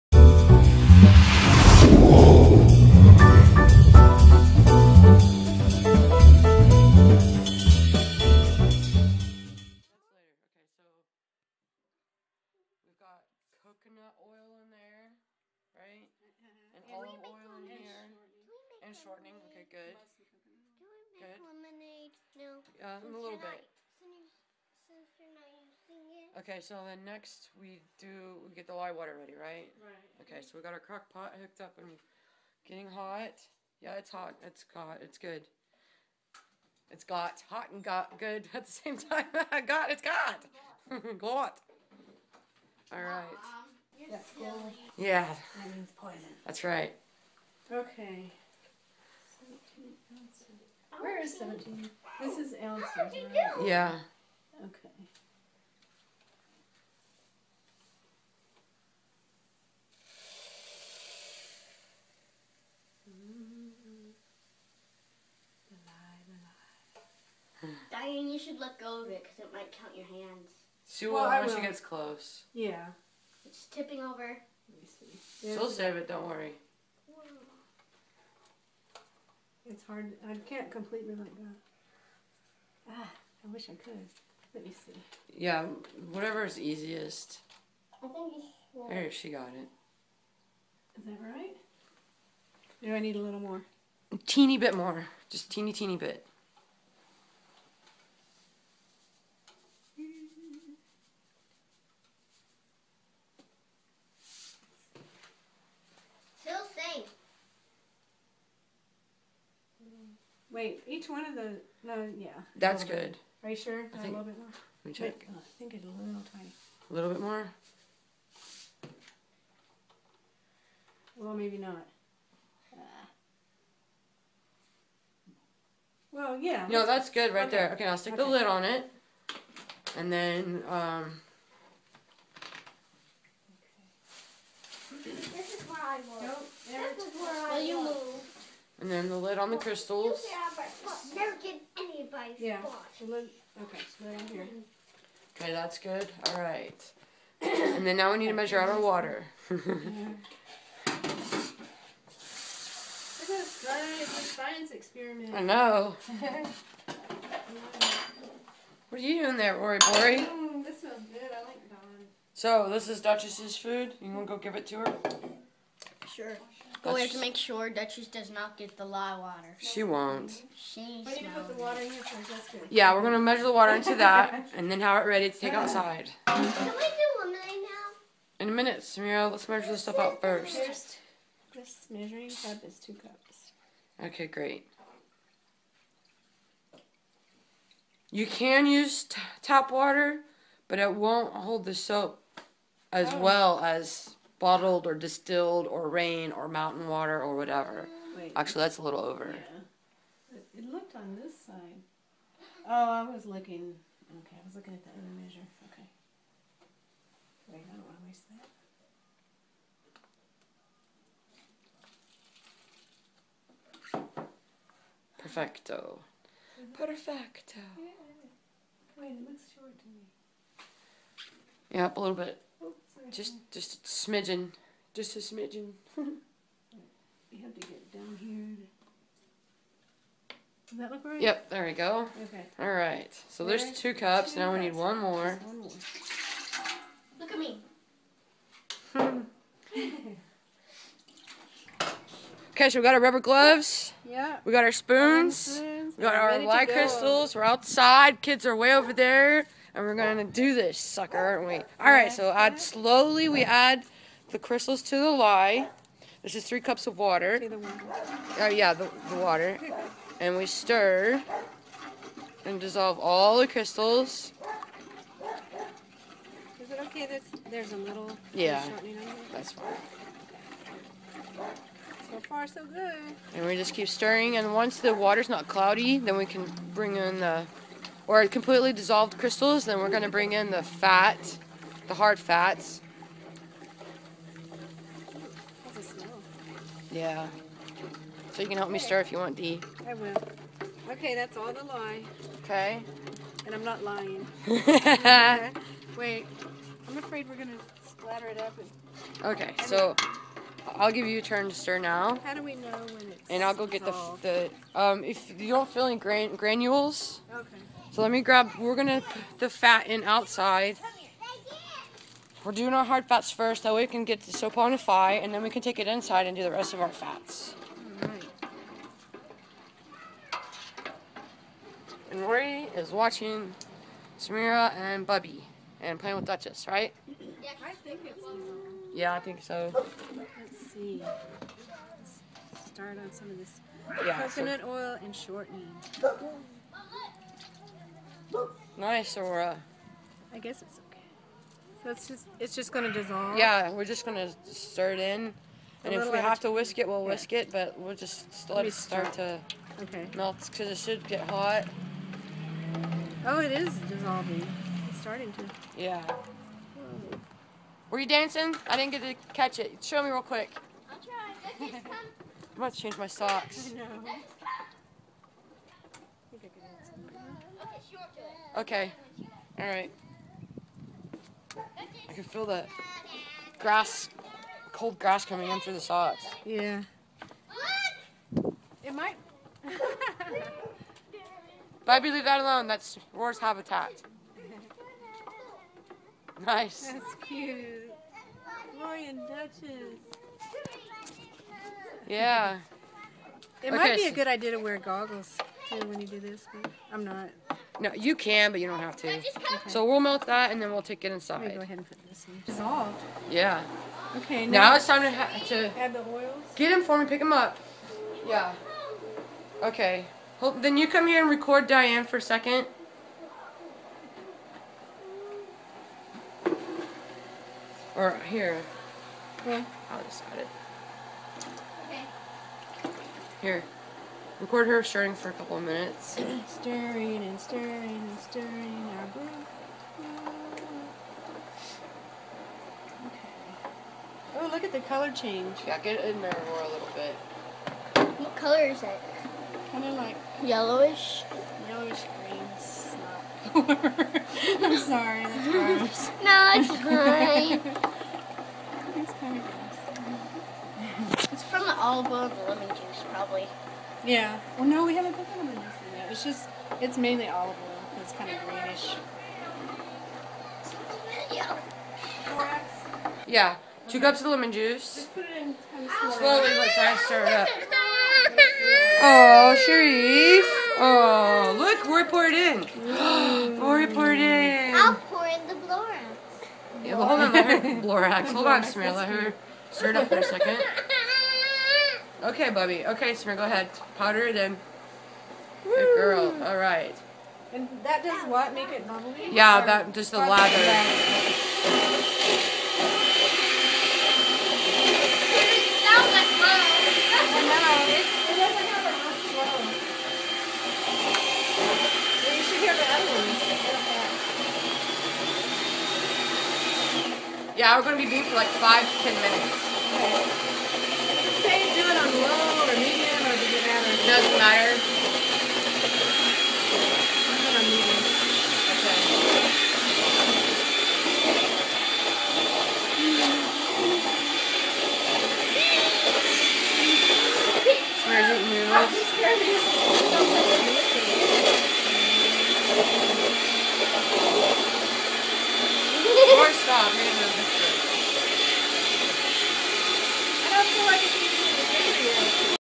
SphinxShow: Making Homemade Lye Soap hot and cold process
Remember kids stay out of the kitchen until soapfonication  process is complete. We made our lye water outside and wore protective gear. We had fun doing this.